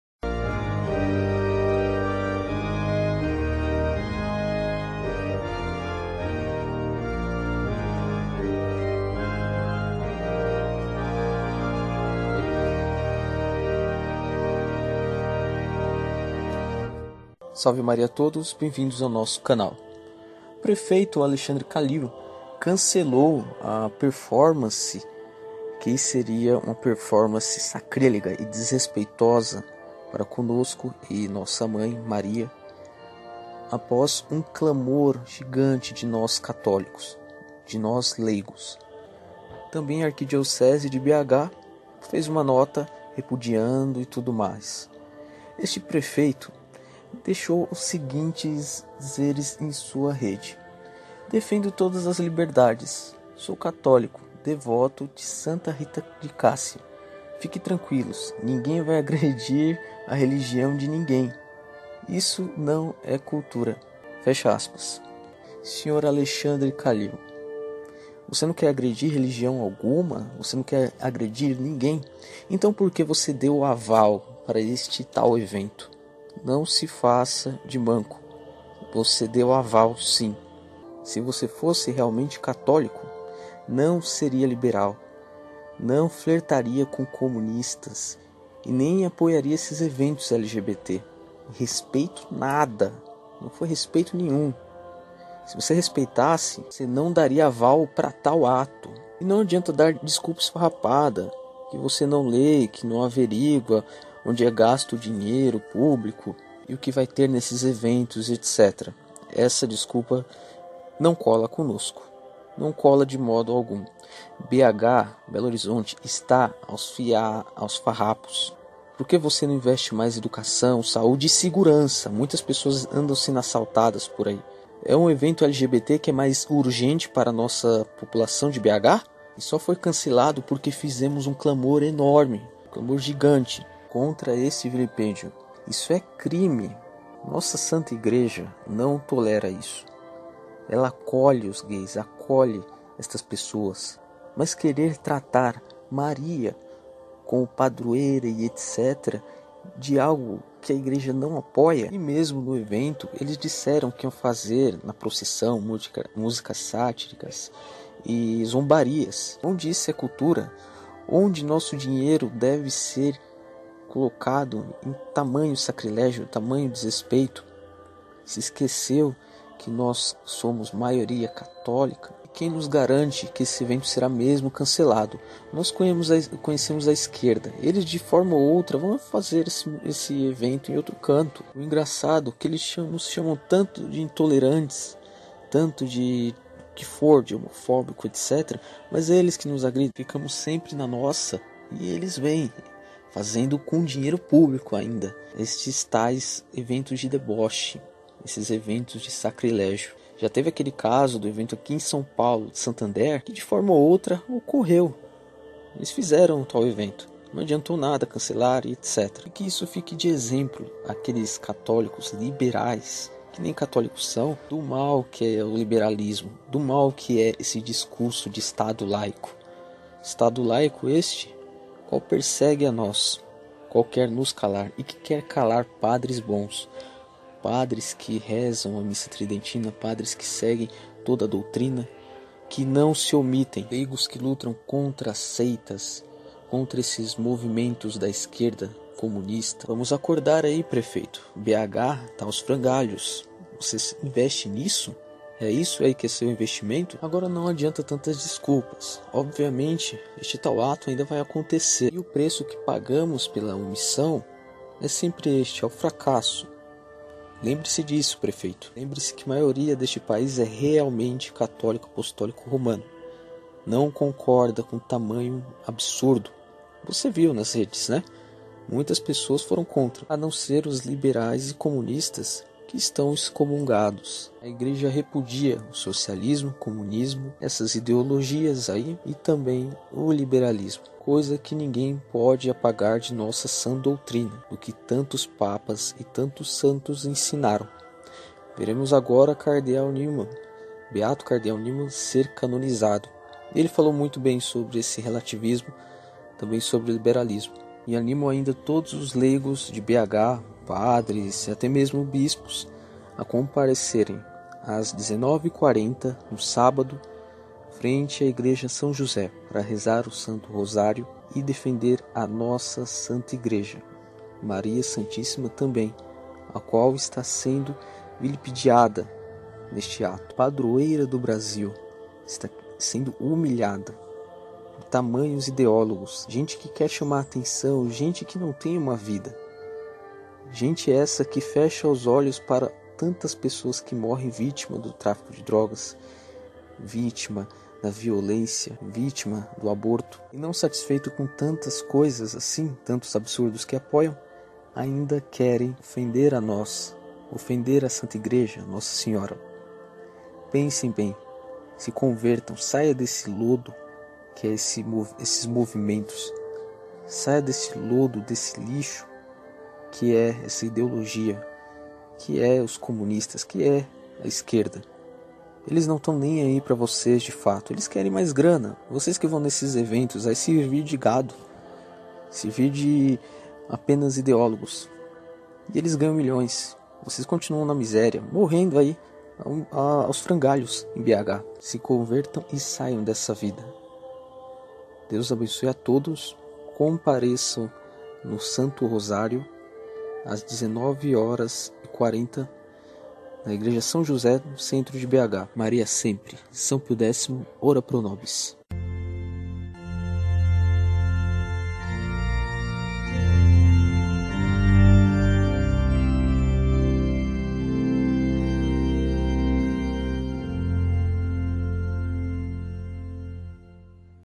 CANCELADO EVENTO SACRÍLEGO! RECADO DE CATÓLICOS A PREFEITO DE BH.
Simples recado ao prefeito, Alexandre Kalil, qual deu aval a evento DESRESPEITOSO E SACRÍLEGO! Nenhuma ofensa sem resposta!
Compareçam no Rosário frente a Igreja de São José, Centro de BH as 19:40 hrs de AMANHÃ.